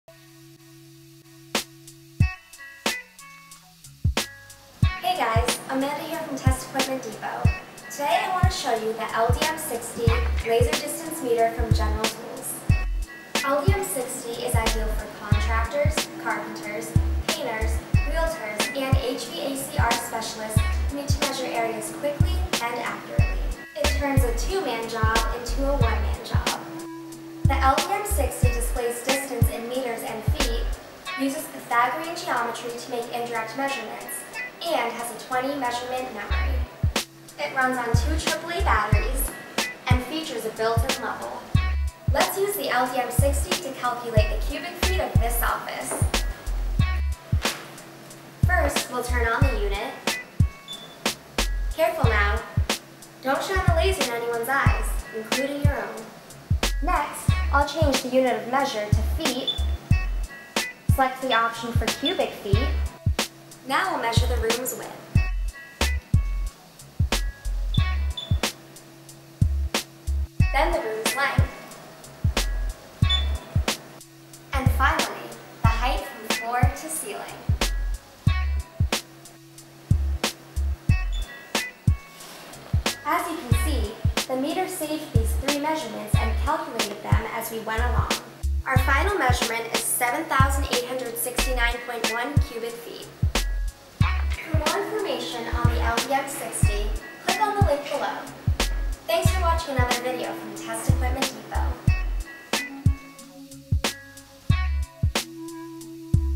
Calculating Cubic Feet with the General Tools LDM60 Laser Distance Meter
The General Tools LDM60 Ideal for contractors, carpenters, painters, realtors and HVAC/R specialists who need to measure lengths, widths, heights, areas and volumes quickly and accurately.